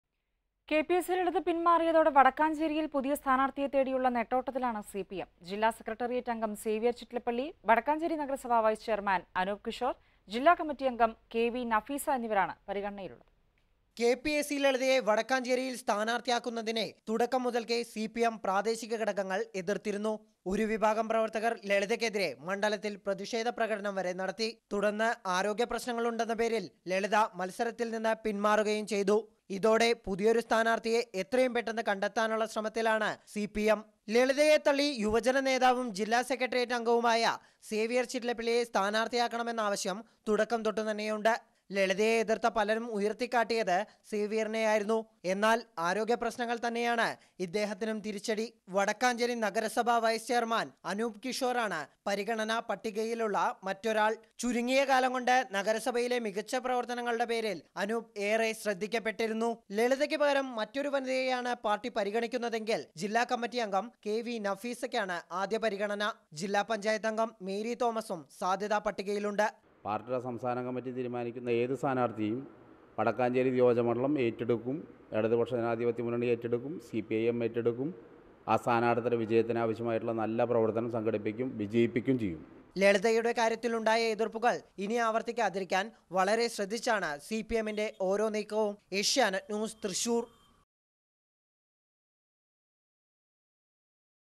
Vadakkanchery candidature issue | Assembly Election 2016
CPIM ) searching new candidate for Vadakkanchery  Assembly constituency

പുതിയ സിപിഎം സ്ഥാനാര്‍ത്ഥിക്കായി വടക്കാഞ്ചേരിയില്‍ അന്വേഷണം സജീവം